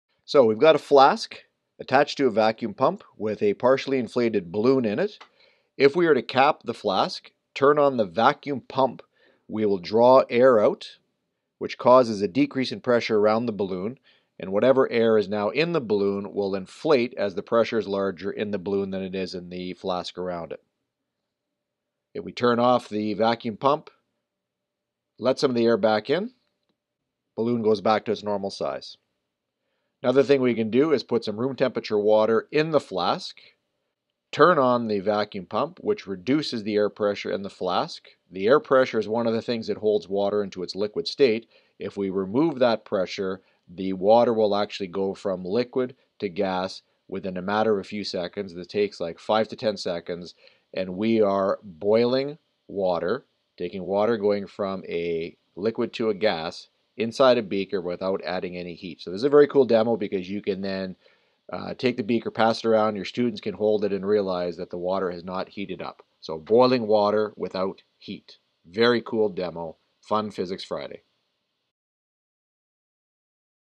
Boiling water without heat ! Water Boiling at room temperature!
Boiling cold water. You can actually "boil" water, or change it from liquid to gas without adding any heat energy.